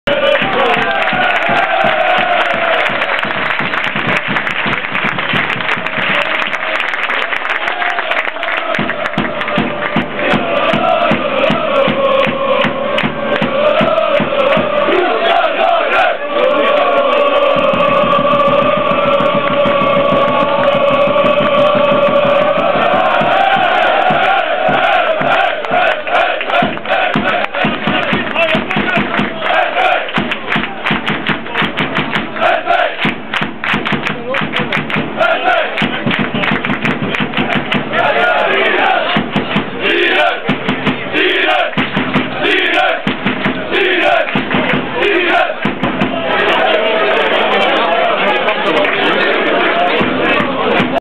opkomen van de spelers ksvr- moeskroen
Waarschijnlijk de laatste keer dat roeselare tegen dit moeskroen speelt. Onze vrienden van dender komen ons deze avond steunen.match trouwens verloren met 1-2.